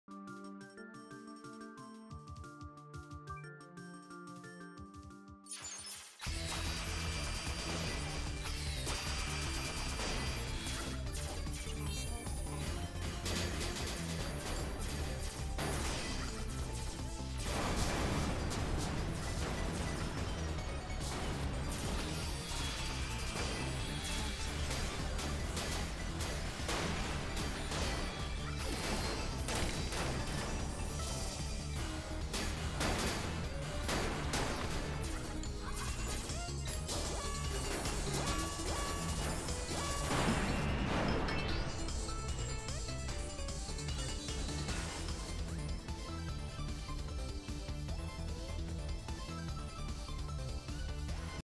9-9 Hyperdimension Y in 34.89 (Savant, Hard)
pretty similar to Boki's strat, but Savant can defeat the Yinimros beneath the start while still reaching Supernova fast enough that the Yinimros in the upper middle area are still gathered. and like normal mode, Savant benefits a lot more from low gravity, and doesn't need high gravity to proceed, so it's best to leave the gravity alone. I also have to make sure I have Eternity Gear for the second delta and the two below it. Eternity Gear isn't bad for the final room either, but Infinity Mind still seems best.